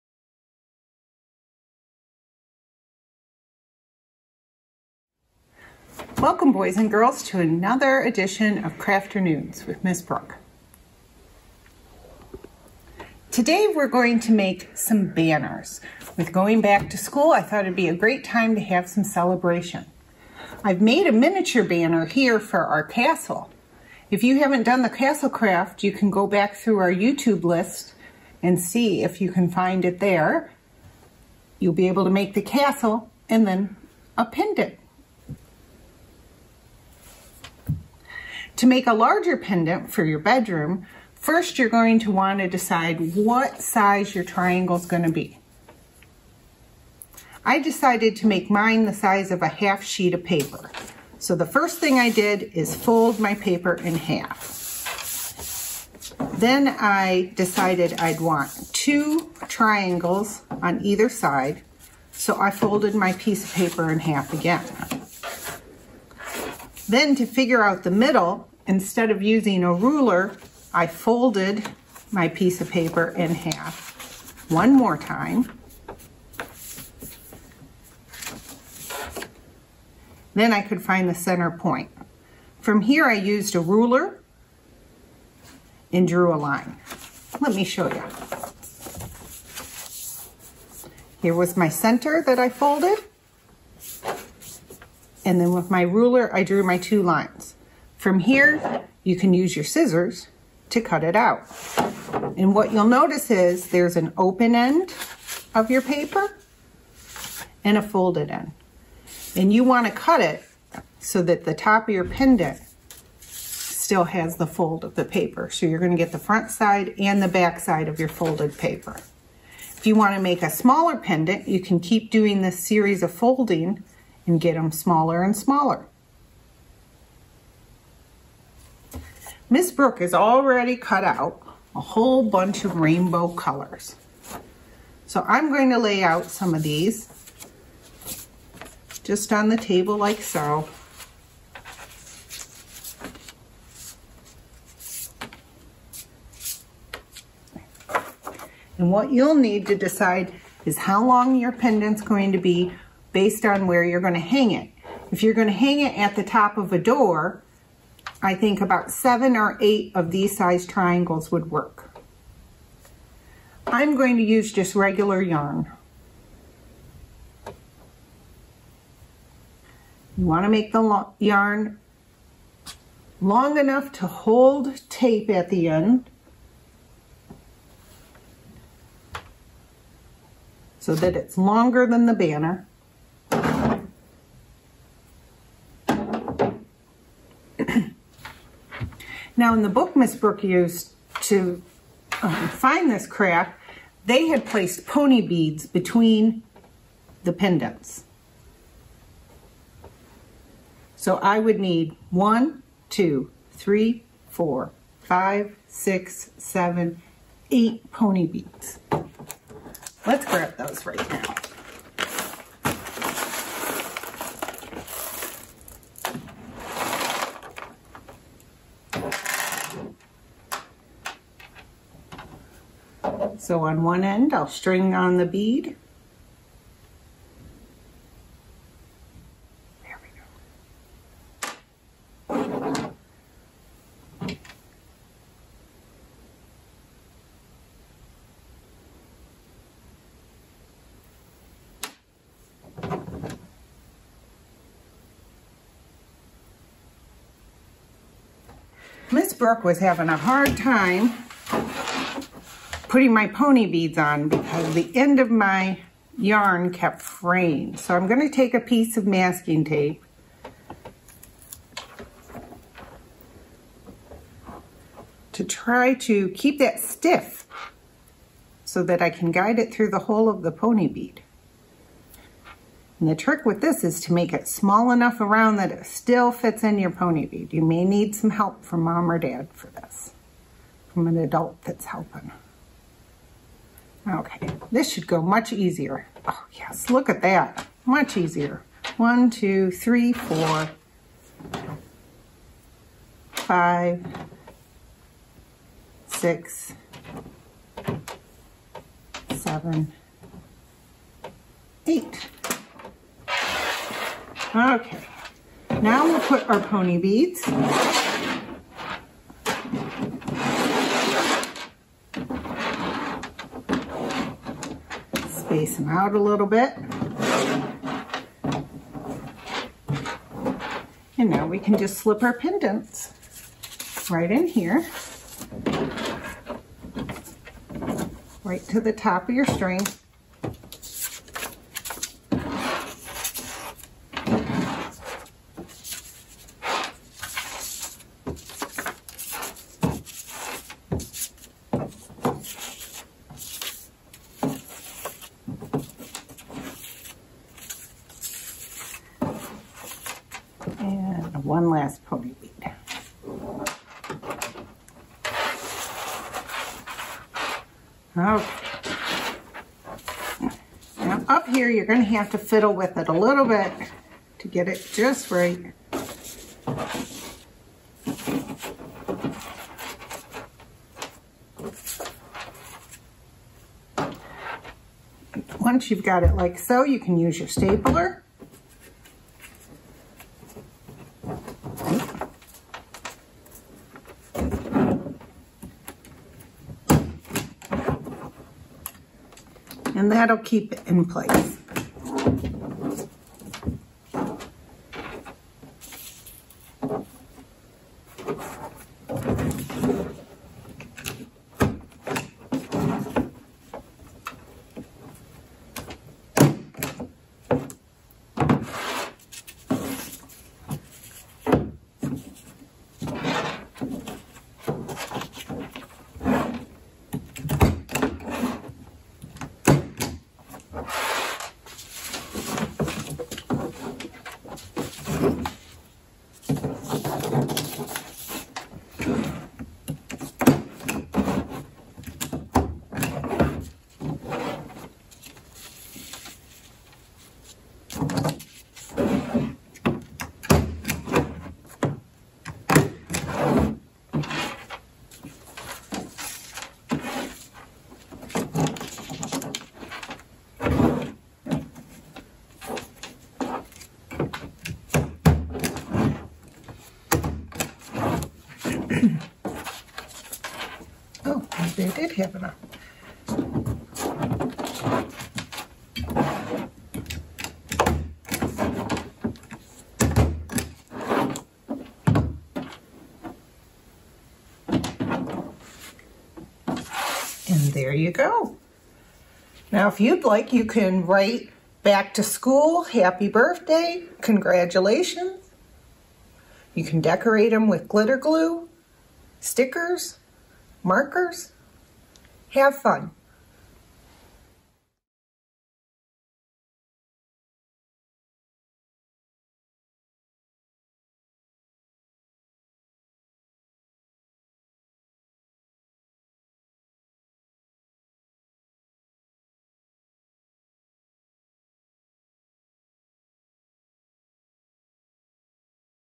Crafternoons - Pennant Banner
Create your own pennant banner with GFJ Library's Brooke!